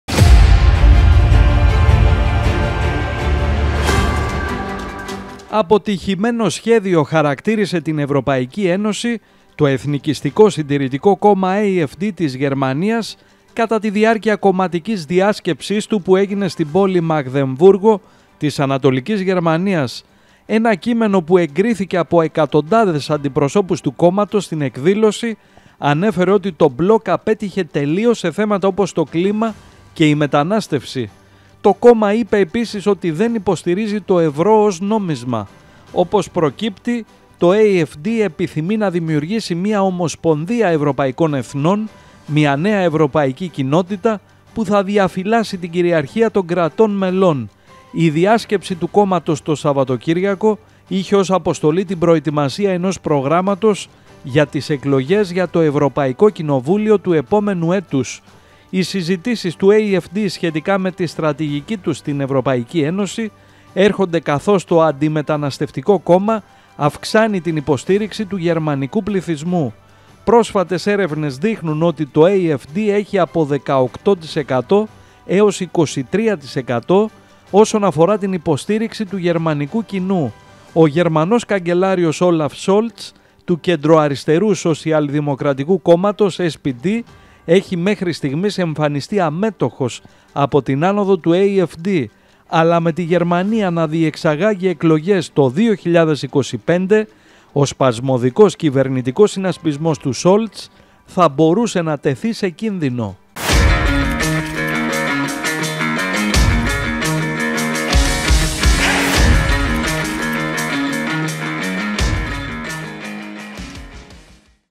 «Η ΕΥΡΩΠΑΪΚΗ ΕΝΩΣΗ ΕΙΝΑΙ ΕΝΑ …ΑΠΟΤΥΧΗΜΕΝΟ ΣΧΕΔΙΟ», ΛΕΕΙ ΤΟ AfD
«Αποτυχημένο σχέδιο» χαρακτήρισε την ΕΕ το Εθνικιστικό – Συντηρητικό κόμμα AfD της Γερμανίας κατά την διάρκεια κομματικής διάσκεψης του που έγινε στην πόλη Μαγδεμβούργο της ανατολικής Γερμανίας. Ένα κείμενο που εγκρίθηκε από εκατοντάδες αντιπροσώπους του κόμματος στην εκδήλωση ανέφερε ότι το μπλοκ «απέτυχε τελείως» σε θέματα όπως το κλίμα και η μετανάστευση . Το κόμμα είπε επίσης ότι δεν υποστηρίζει το ευρώ ως νόμισμα.